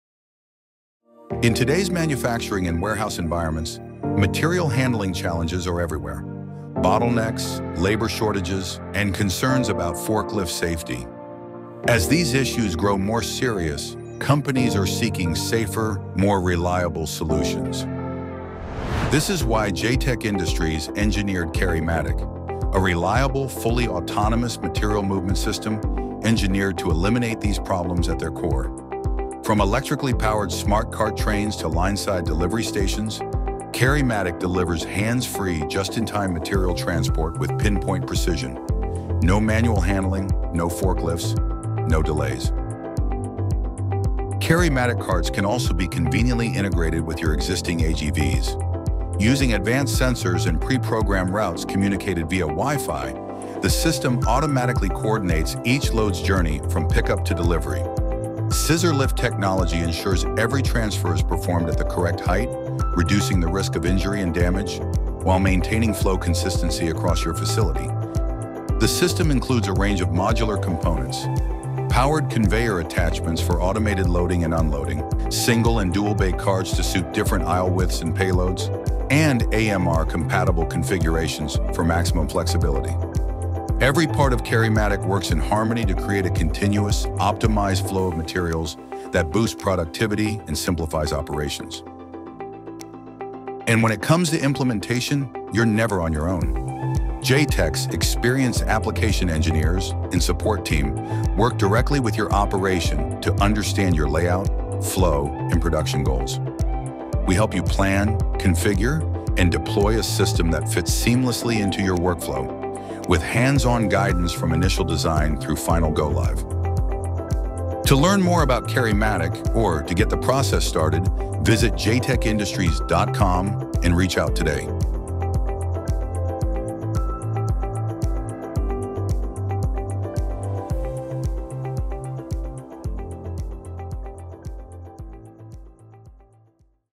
How Automated Material Handling Works — CarryMatic from Jtec Industries
In today’s fast-paced manufacturing and warehouse environments, material handling challenges—like bottlenecks, labor shortages, forklift congestion, and rising safety risks—can slow you down. That’s why CarryMatic by Jtec Industries is the smart, scalable next step in automated material handling.

✅ Fully autonomous transport system
✅ Compatible with AGVs and manual tuggers
✅ Includes powered conveyor carts, scissor lifts & lineside stations
✅ Hands-free, just-in-time material movement
✅ Optimized routes via Wi-Fi communication
✅ Reduces injuries, delays, and dependency on forklifts

Our modular system of industrial trailer carts includes single and dual-bay configurations, AMR-compatible solutions, and built-in safety sensors to ensure damage-free, ergonomic transfers every time. Whether you’re upgrading your current process or building a fully automated workflow, CarryMatic delivers unmatched efficiency and reliability.

Need help getting started? Jtec’s experienced application engineering and support team walks with you from initial planning to full deployment, ensuring seamless integration with your facility’s operations.

🔧 Learn more about Jtec’s automated trailer carts, powered industrial carts, and warehouse automation systems